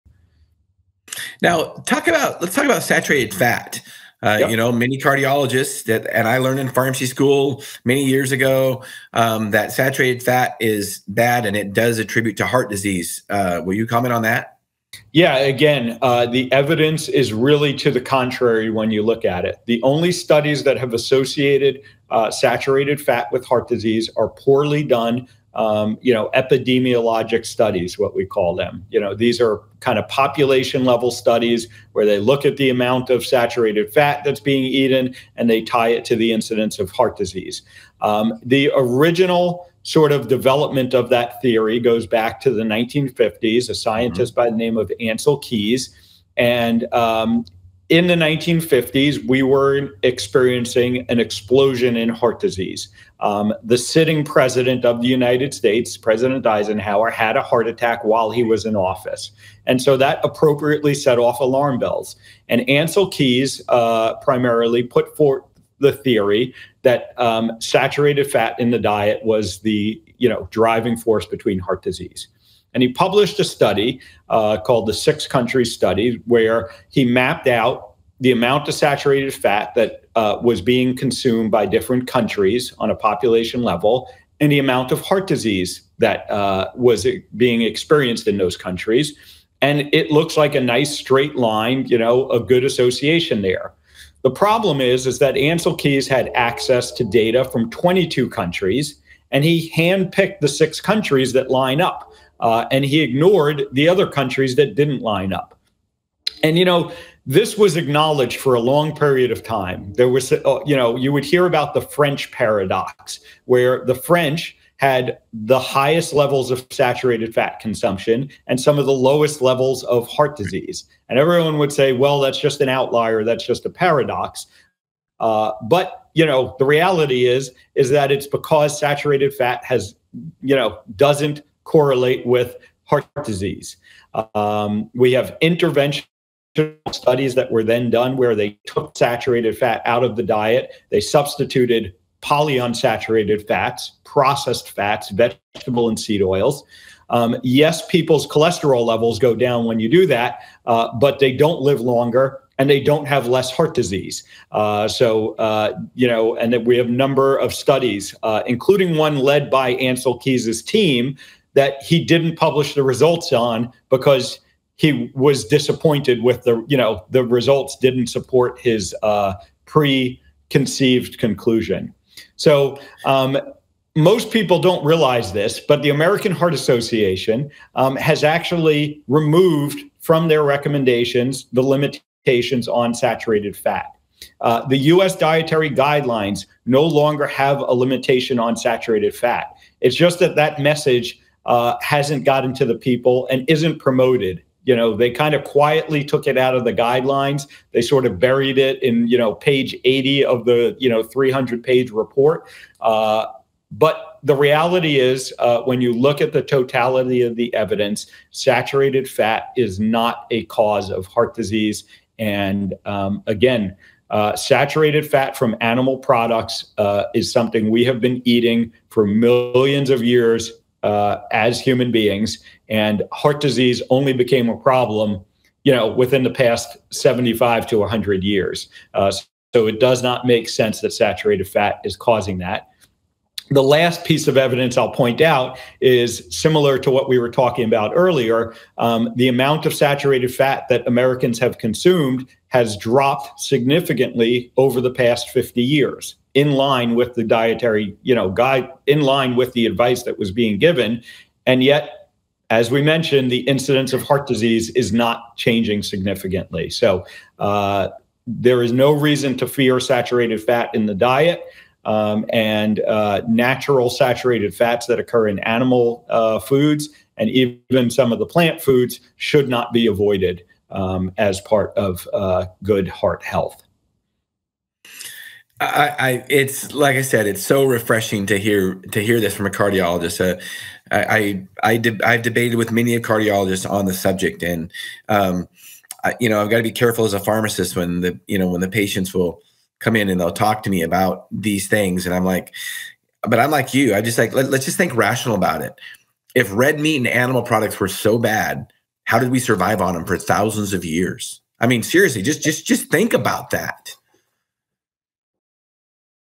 Saturated Fat is not Bad for You with Dr. Philip Ovadia and Shawn Needham, R. Ph.
"The only studies that have associated saturated fat with heart disease are poorly done."

On this clip of 333 of Health Solutions, Shawn Needham, R. Ph. discusses how saturated fat is not bad for you. Dr. Ovadia talks about the poorly done studies that told us saturated fat is tied to heart disease.

Google Podcasts ~ search Health Solutions with Shawn & Janet